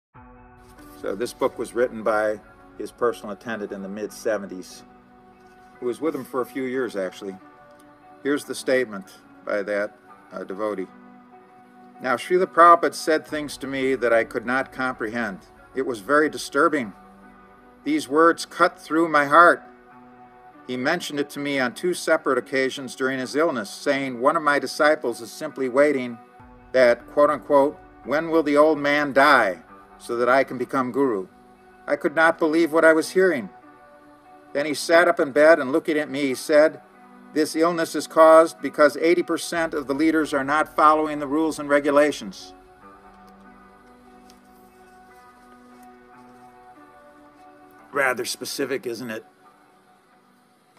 "ISKCON" & The Vitiated G.B.C.
“Regular guru, that’s all.” The G.B.C. defied this. Also, it never informed Prabhupäda that it would not hold elections in 1973, although its founding charter demanded those elections. Deviation by omission–and deviation by commission when it divided the world into eleven zones for the benefit of eleven great pretenders. And remember that all of them were members of the Commish.

KCd's Missive original source: Lets Play "28 IF" - part 1 - 28. June, 2016